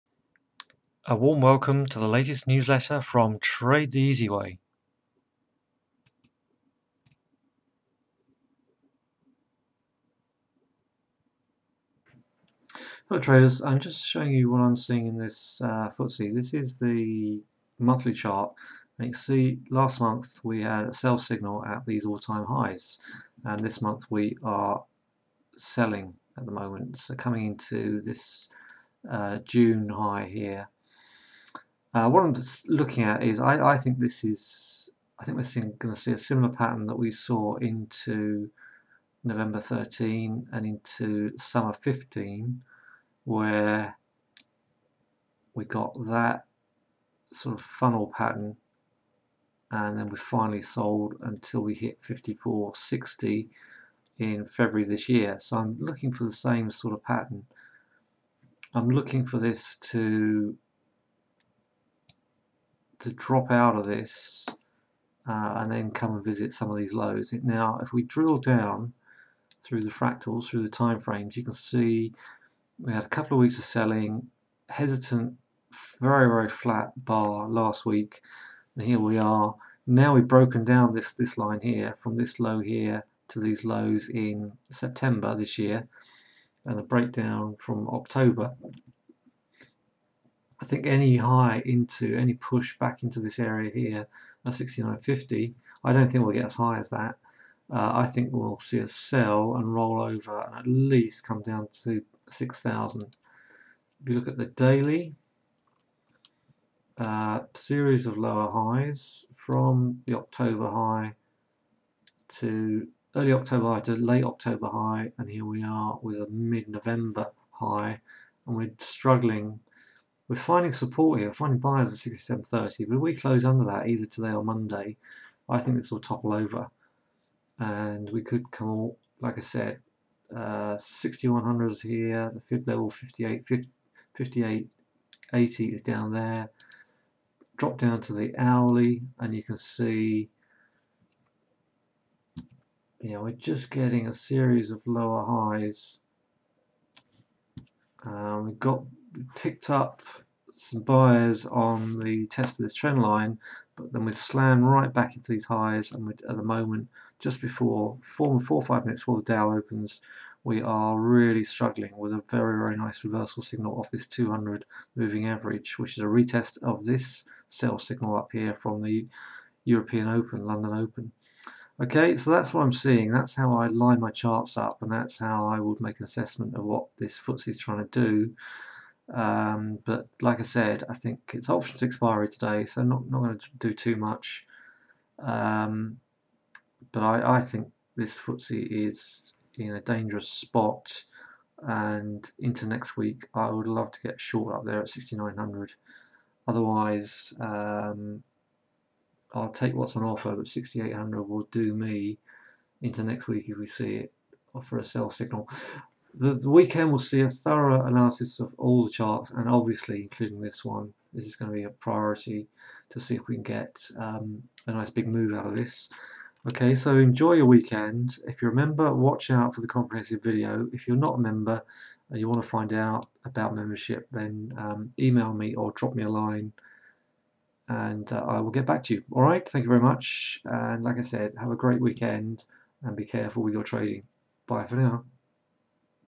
FTSE - big picture
The FTSE has held up well despite claims the UK would suffer post Brexit.
But politics aside, what do the charts tell us about the possible future direction of the FTSE?
Here I talk about the big picture, the charts on the big timescales and the pattern I see emerging for the FTSE.

More on this in the weekend members' video and Traders' Round Table session on Monday.

Mike